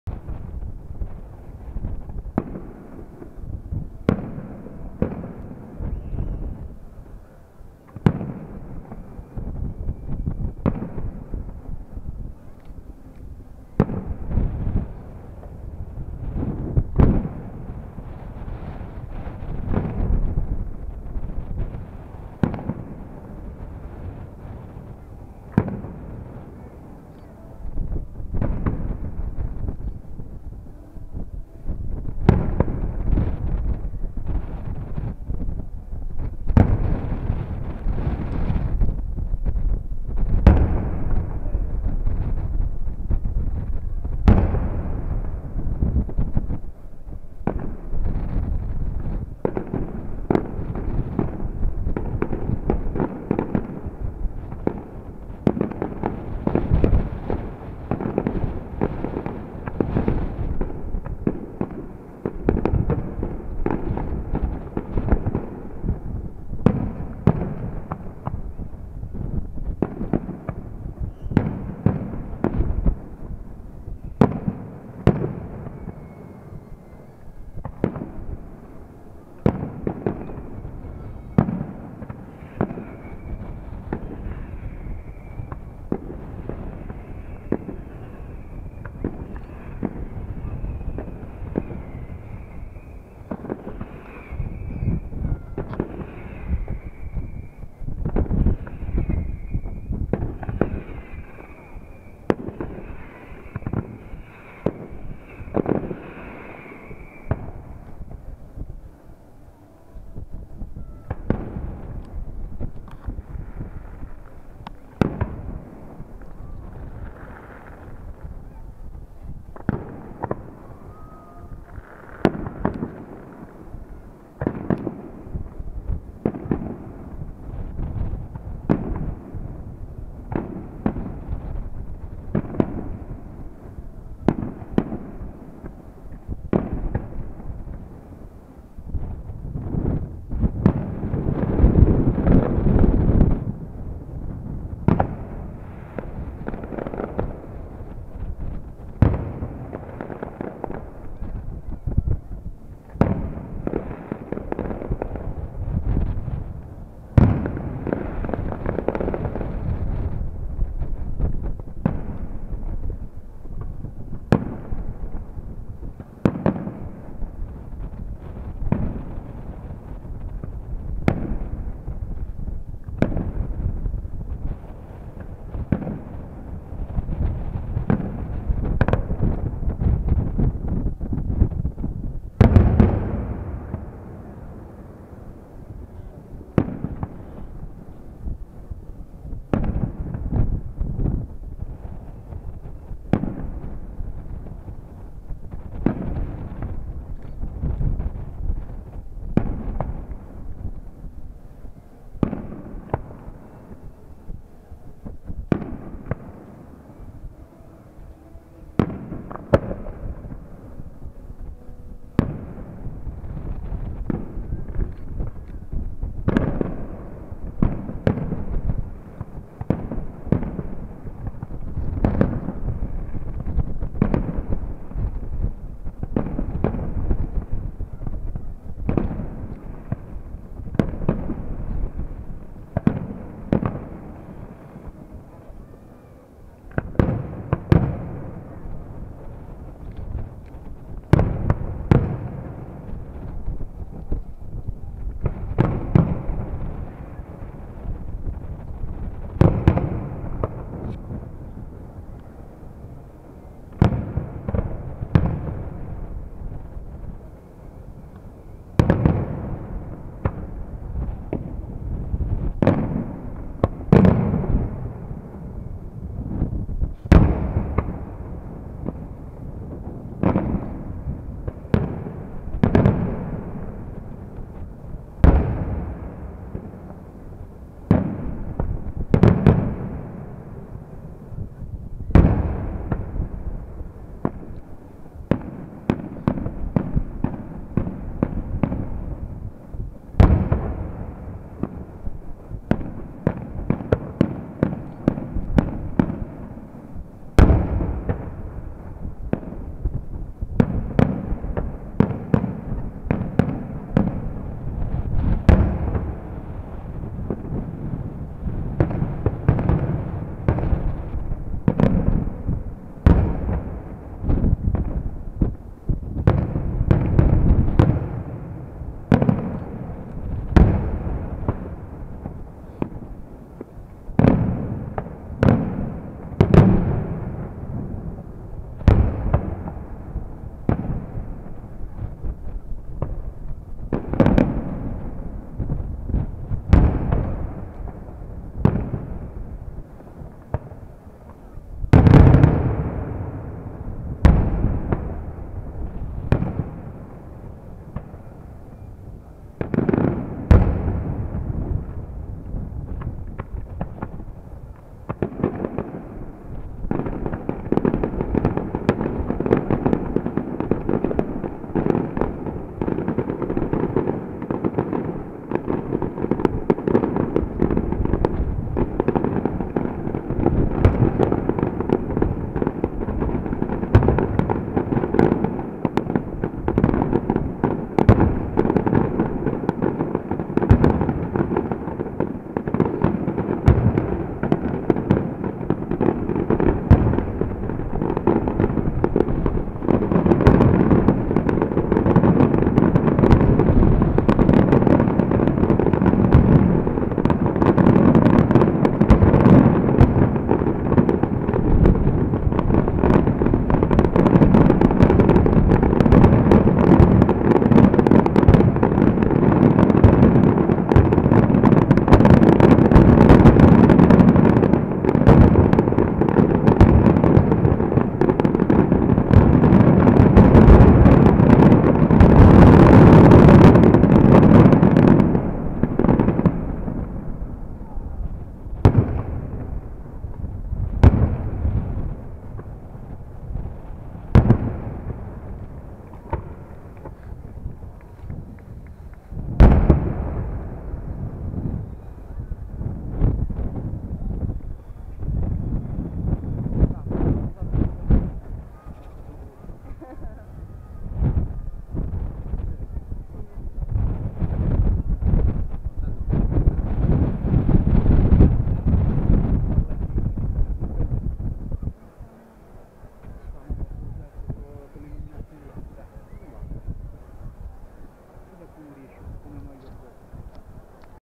Fuochi Festa Patronale "Madonna delle Grazie" 2015 Palagianello (TA)
Per tutti i Palagianellesi e non che non hanno vissuto in prima persona l'emozione perché siete distanti dal nostro piccolo paesino vi propongo la visione dei fuochi pirotecnici di fine festa.